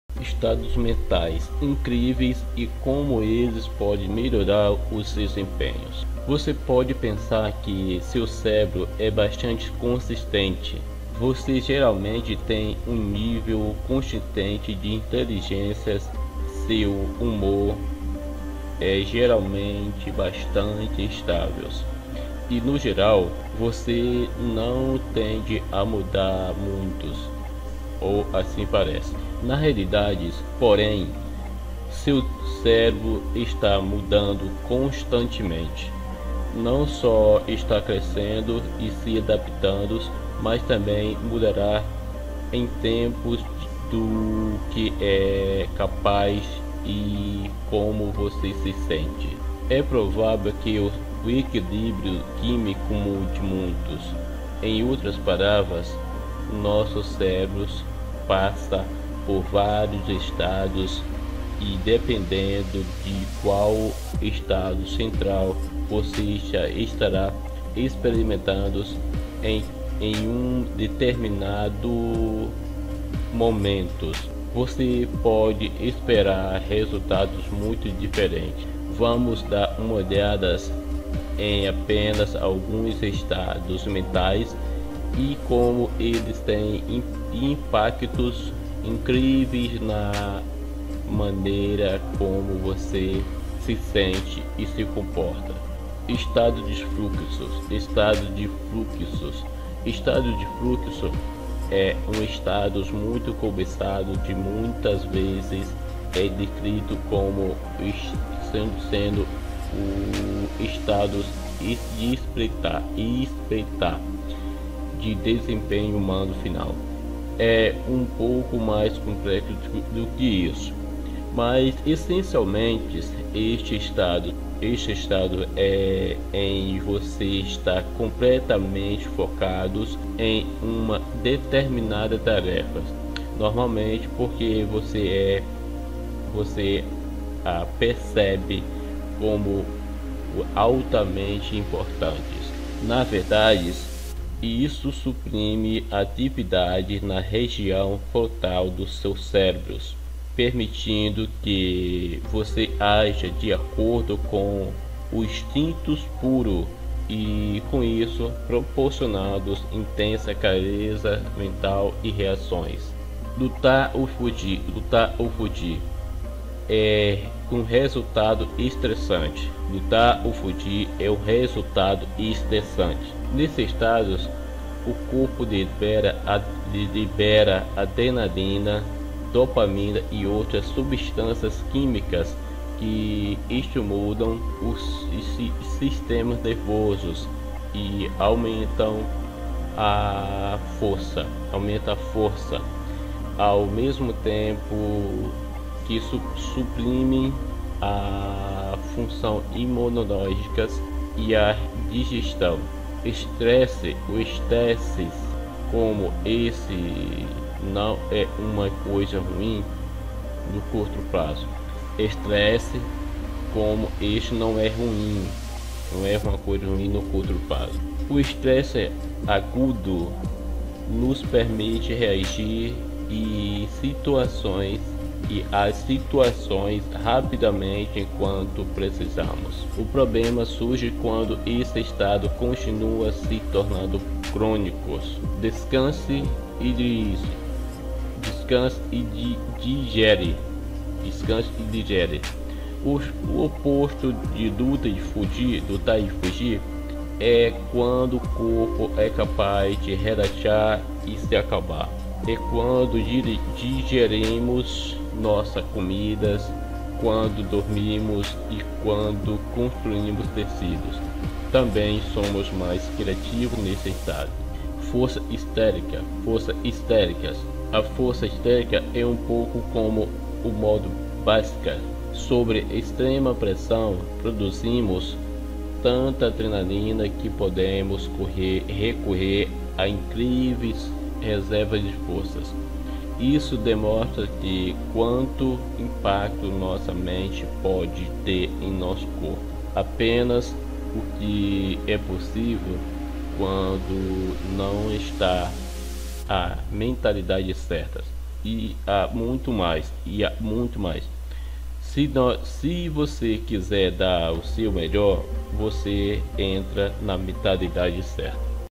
Estados mentais incríveis e como eles podem melhorar o desempenho
Você pode pensar que seu cérebro é bastante consistente.
Você geralmente tem um nível consistente de inteligência, seu humor é geralmente bastante estável e, no geral, você não tende a mudar muito. Ou assim parece.
Na realidade, porém, seu cérebro está mudando constantemente.
Não só está crescendo e se adaptando, mas também mudará em termos do que é capaz e como você se sente.
É provável que o equilíbrio químico mude muito.
Em outras palavras, nosso cérebro passa por vários estados e dependendo de qual estado cerebral você está experimentando em um determinado momento, você pode esperar resultados muito diferentes.
Vamos dar uma olhada em apenas alguns estados mentais e como eles têm impactos incríveis na maneira como você se sente e se comporta.

Estado de Fluxo
Um estado de fluxo é um estado muito cobiçado que muitas vezes é descrito como sendo o estado de "espreitar" o desempenho humano final.
É um pouco mais complexo do que isso.
Mas essencialmente, este é um estado em que você está completamente focado em uma determinada tarefa, normalmente porque você a percebe como altamente importante.
Na verdade, isso suprime a atividade na região frontal do cérebro, permitindo que você aja de acordo com o instinto puro e, com isso, proporcionando intensa clareza mental e reações.

Lutar ou fugir
Lutar ou fugir é um estado de estresse.
Nesse estado, o corpo libera adrenalina, dopamina e outras substâncias químicas que estimulam o sistema nervoso e aumentam a força, ao mesmo tempo que suprimem a função imunológica e a digestão.
Estresse como esse não é uma coisa ruim no curto prazo.
O estresse agudo nos permite reagir às situações rapidamente quando precisamos.
O problema surge quando esse estado continua e se torna crônico.

Descanse e digere
O oposto de lutar ou fugir, é quando o corpo é capaz de relaxar e se acalmar.
É quando digerimos nossa comida, quando dormimos e quando construímos tecidos.
Também somos mais criativos neste estado!

Força histérica
A força histérica é um pouco como o "modo beserker".
Sob extrema pressão, produzimos tanta adrenalina que podemos recorrer a incríveis reservas de força.
Isso demonstra quanto impacto nossa mente pode ter em nosso corpo - apenas o que é possível quando você está com a mentalidade certa.
E há muito mais.
Se você quiser dar o seu melhor, é melhor entrar na mentalidade certa!